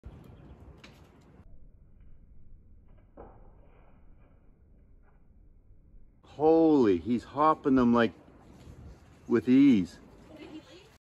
Can Coyotes Climb Fences? Watch This!
Gates Wildlife Field Supervisor Joel caught a coyote jumping a fence in Ajax, Ontario last week. Watch how easily the coyote makes the jump to the top of a 6 foot fence.